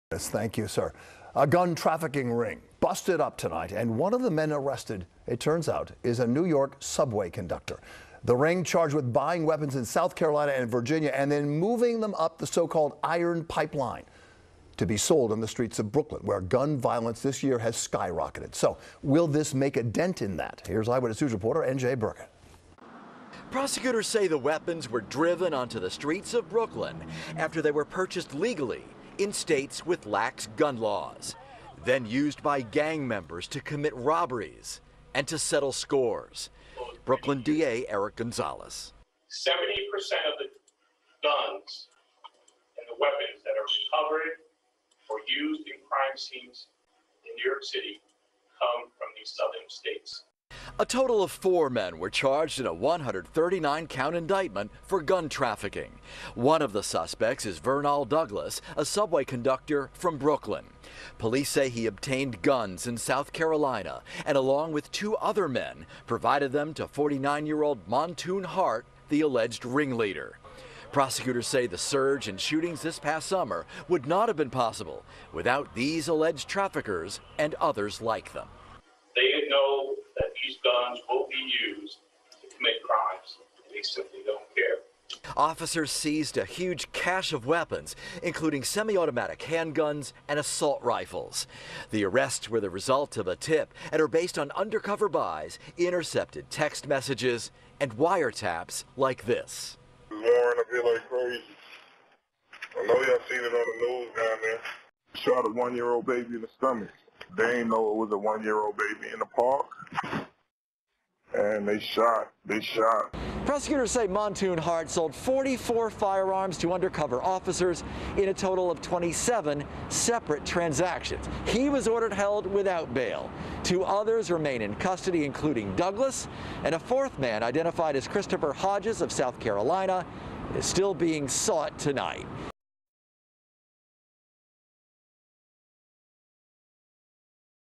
NYPD busts alleged gun trafficking ring
Brooklyn District Attorney Eric Gonzalez and NYPD Commissioner Dermot Shea on Thursday announced the bust of an alleged gun trafficking ring.

They say members of the ring purchased firearms in South Carolina and Virginia that were then transported on the Iron Pipeline and destined to be sold on the streets of Brooklyn.

Gonzalez said the investigation spanned from October 2019 to October 2020 and began based on information obtained about an individual who was allegedly known as a gun merchant in Brownsville.

NEW HERE? –